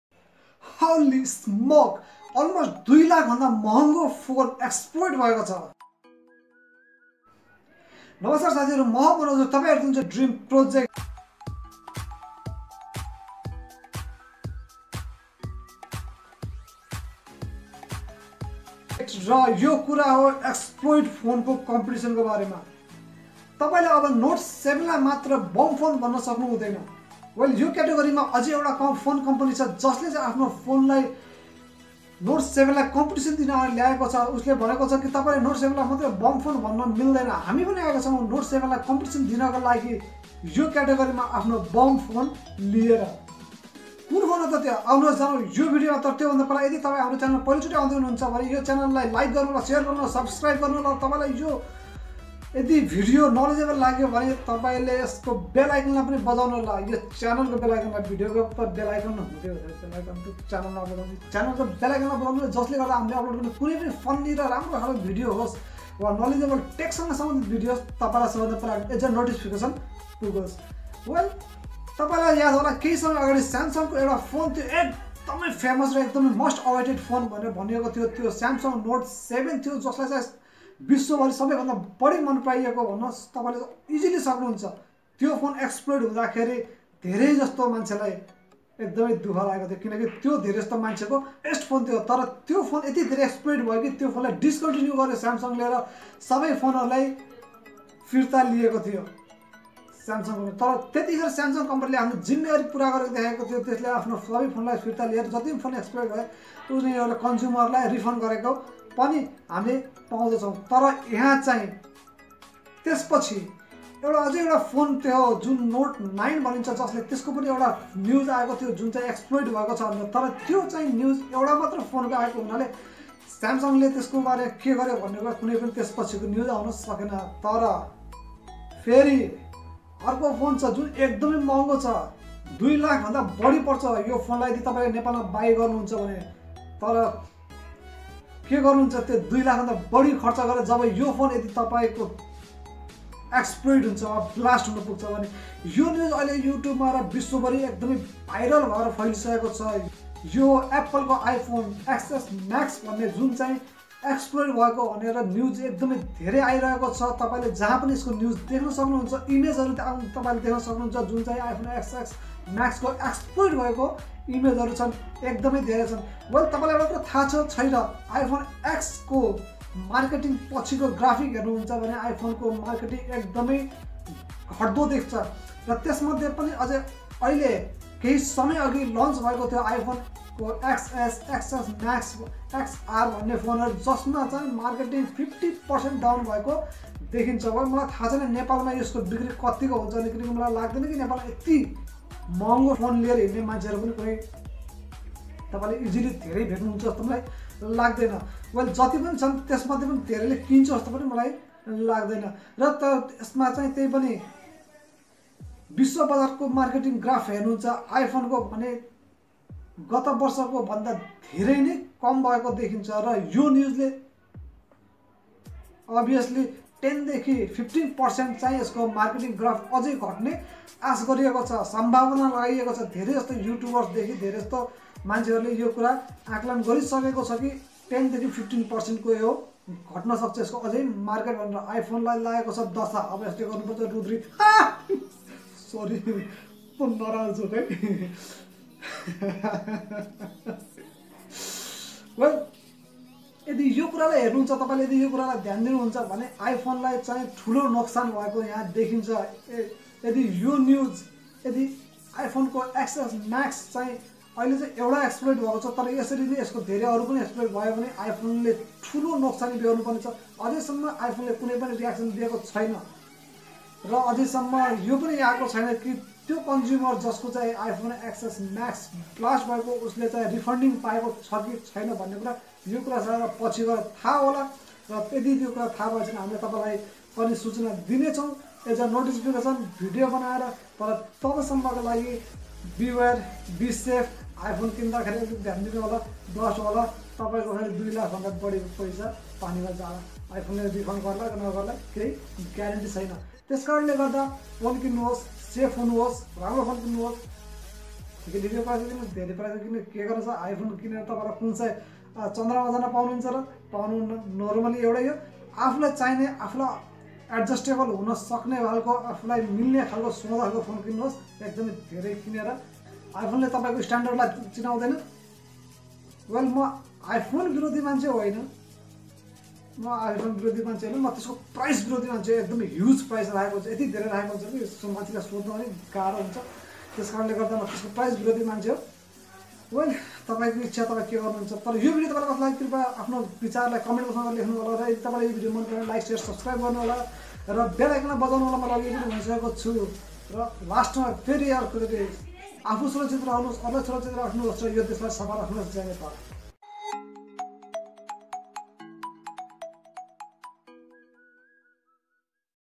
iphone xs max explode ll iphone xs max brust
Almost 2 lac price mobile gone explode iphone xs max is more expensive than previous iphones but iphone xs max is another phone on explode list